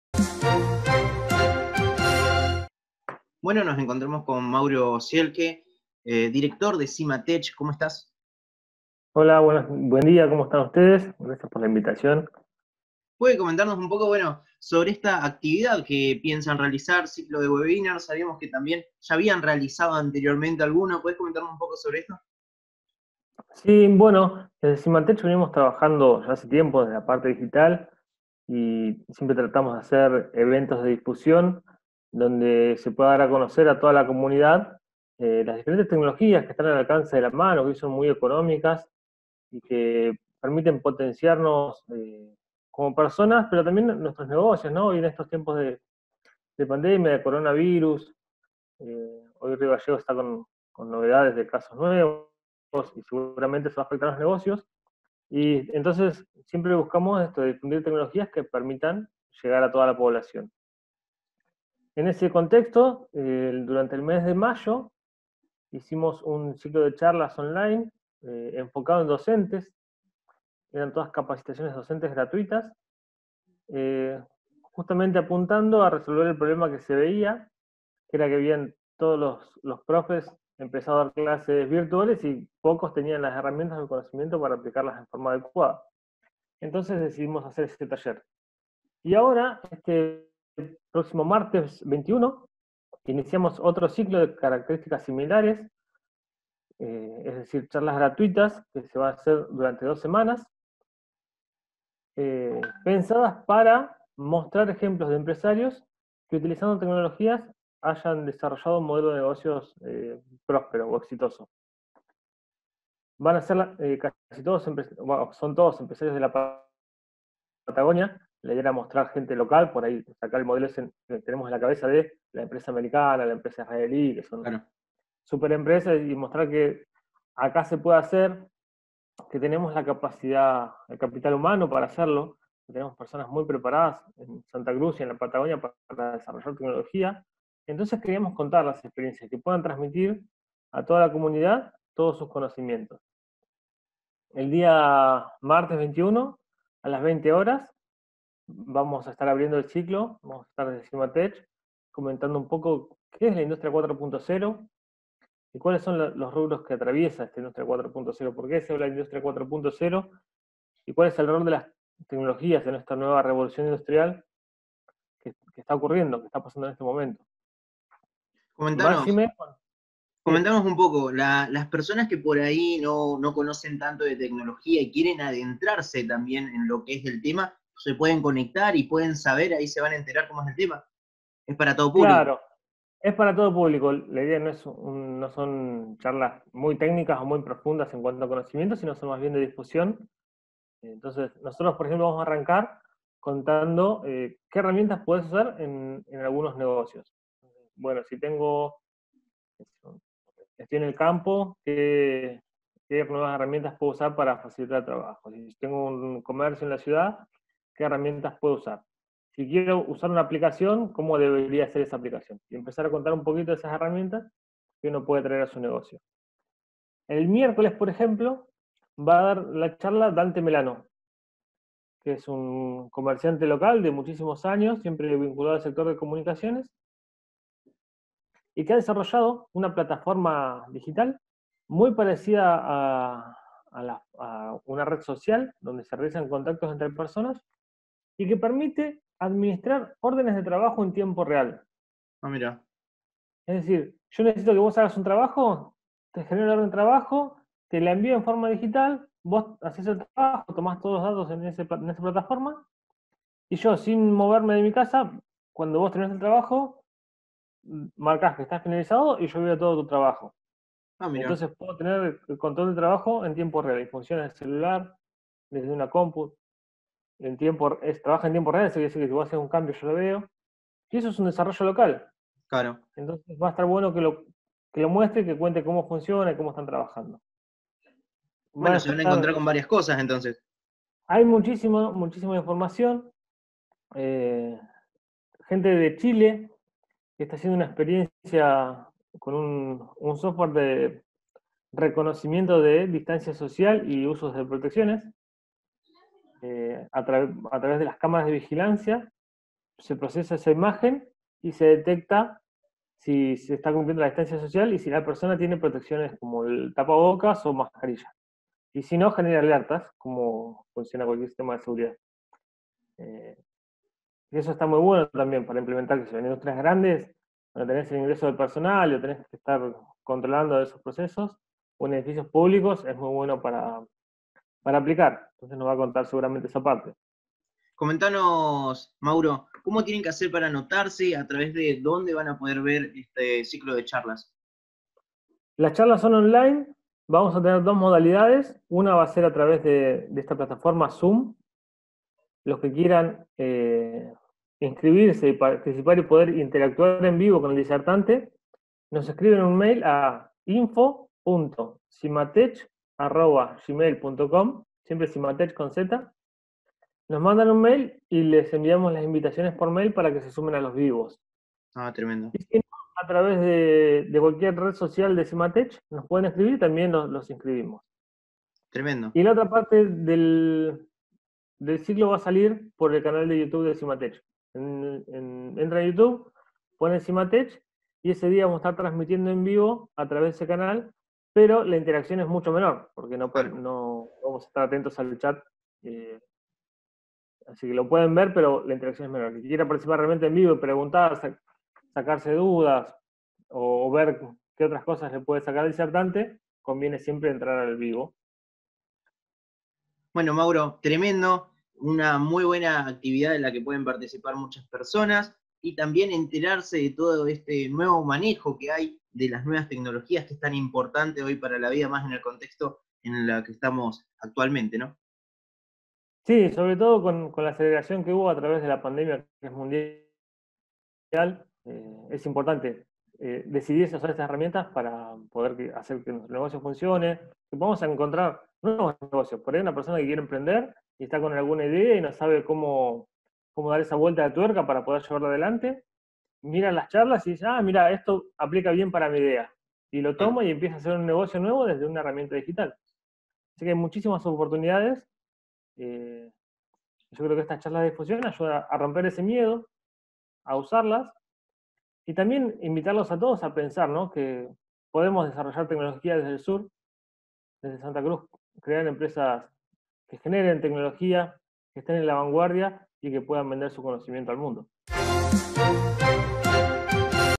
Mauro Ziehlke - Se realizará Ciclo de Webinars sobre "Tecnologías Disruptivas"
Conversamos con Mauro Ziehlke, Director de Zimatech, quién nos comentó sobre el Ciclo de Webinars sobre tecnologías disruptivas que realizarán del 21 al 31 de julio.